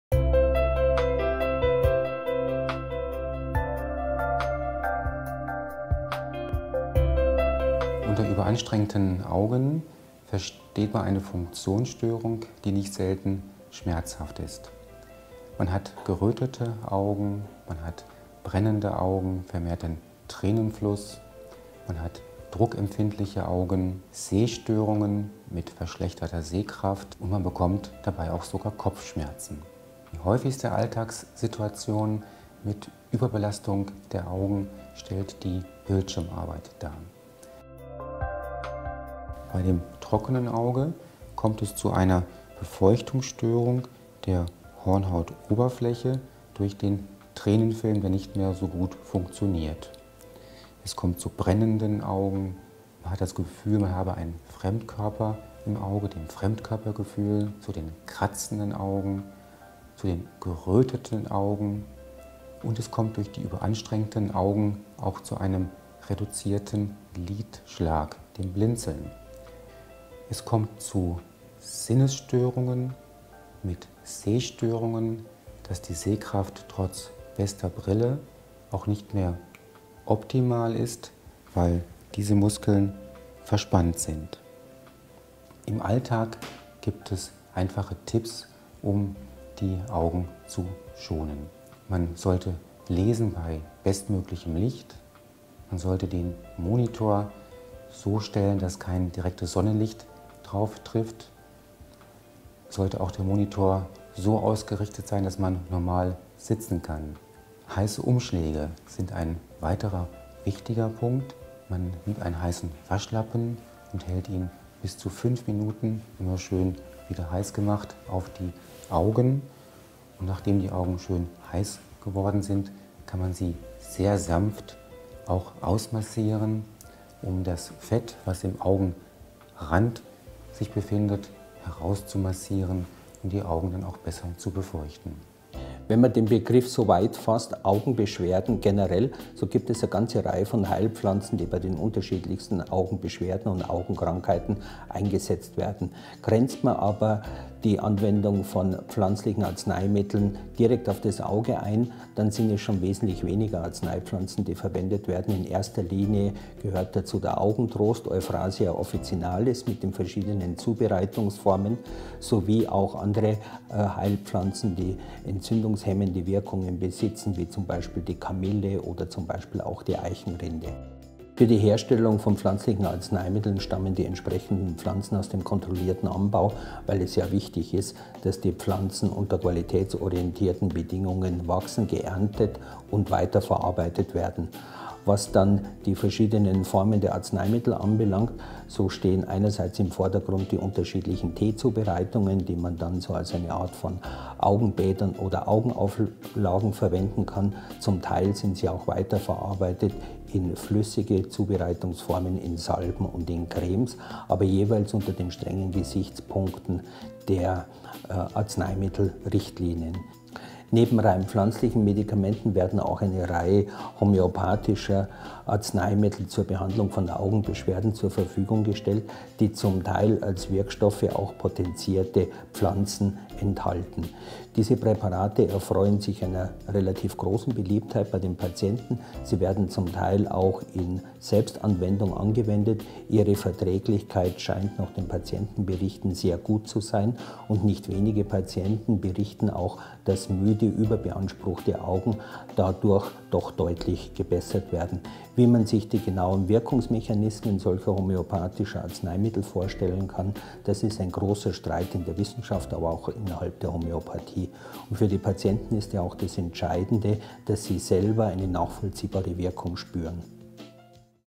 Das überanstrengte Auge
Wussten Sie, dass Sie Ihr Auge bei Überanstrenung schädigen? Die Tipps vom Experten helfen bei der Entspannung der Augen...